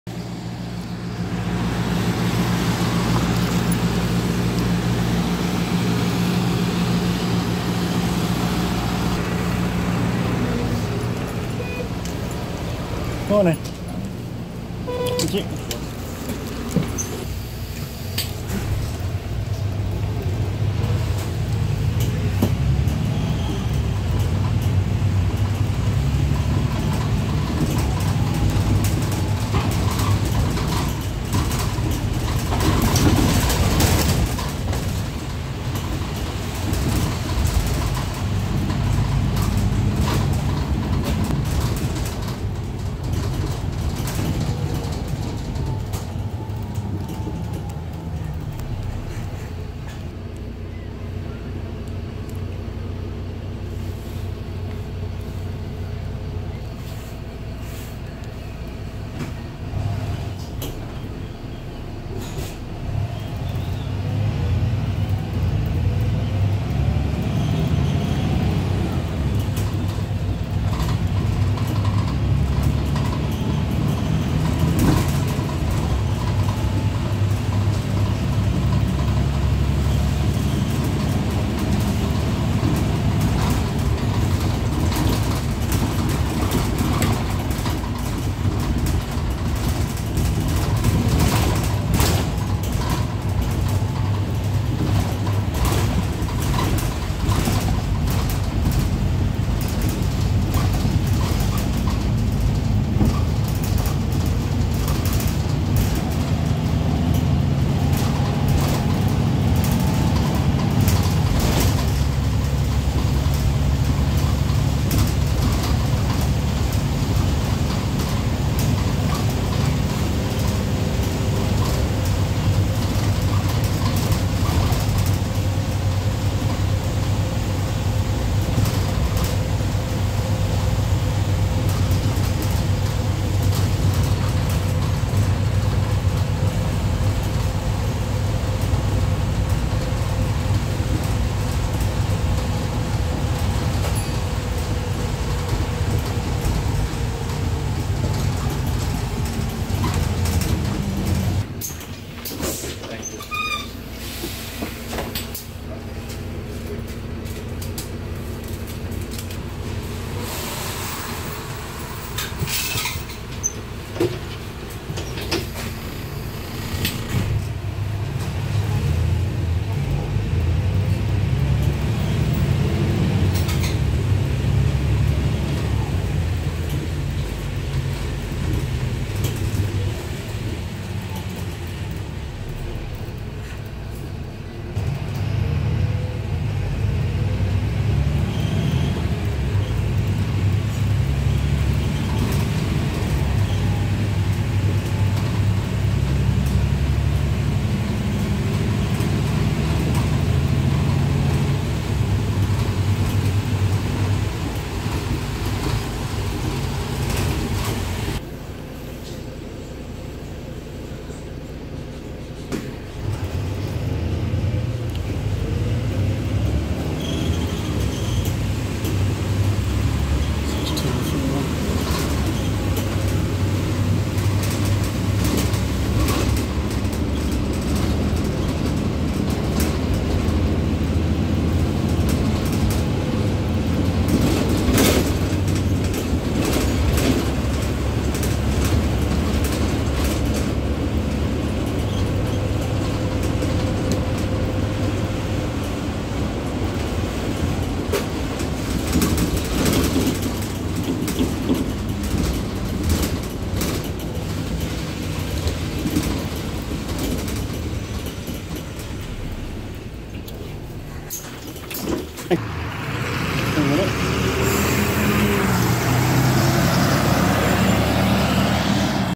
Awsome 41 go ahead northwest mercdez benz citario fleet 6231 manchester vic to queen road /motgm
Awsome 41 go ahead northwest mercdez benz citario fleet 6231 manchester vic to queen road /motgm  @MoTGM
Only decent buses go north west have in greater manchester